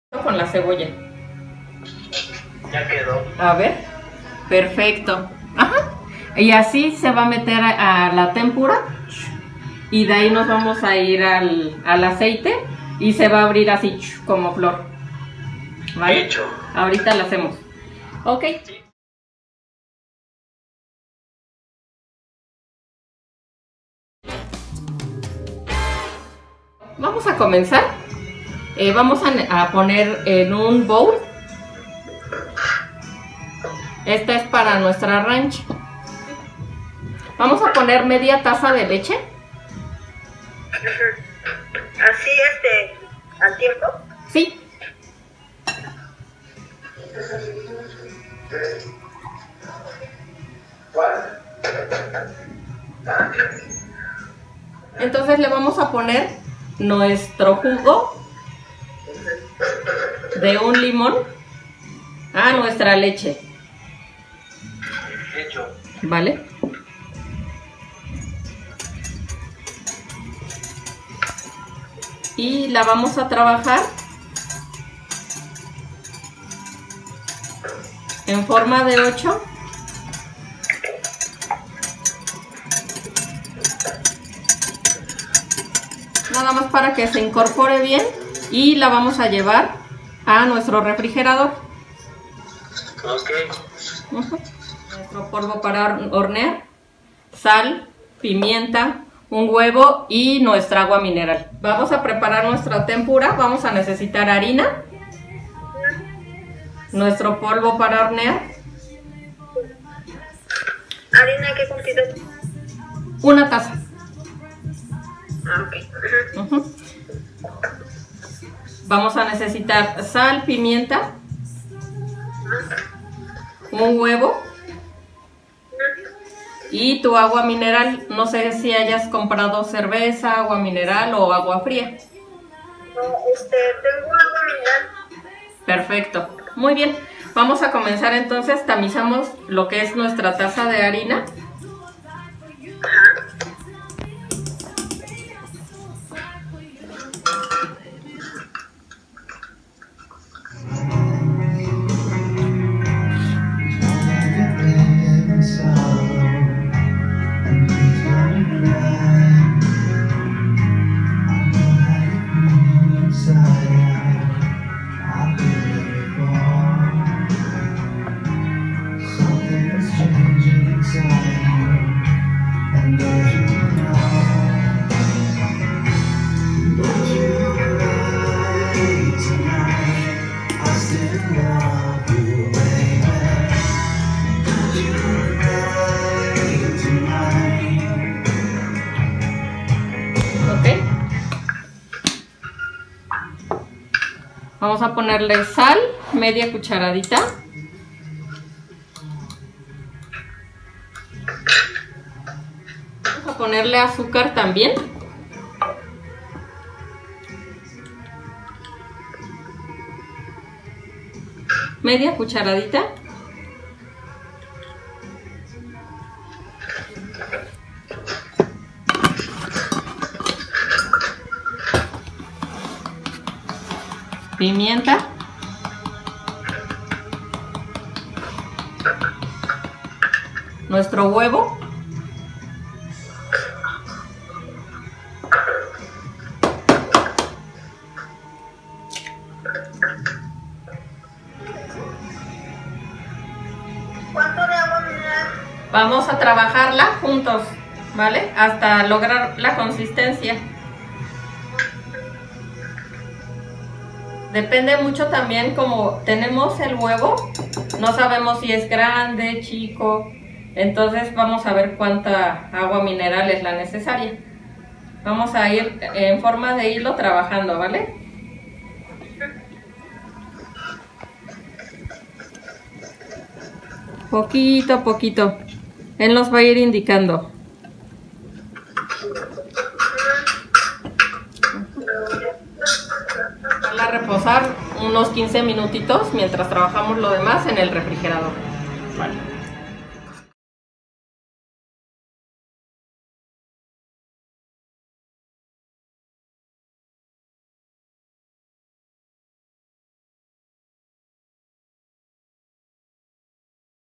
TEMPURA Y RANCH RECETA | Clase en línea de Botanas para eventos deportivos | Crudités y Alitas Ranch
Te Compartimos la Manera de Impartir una Clase de Cocina en Línea, Elaborando Botas para los Mejores Eventos Deportivos. Aderezo Ranch para Alitas y Crudités, así como la Masa de Tempura para Cebolla en Flor y Aros de Cebolla en Tempura. Recetas que seguramente podrás adoptar para compartir con familiares y amigos.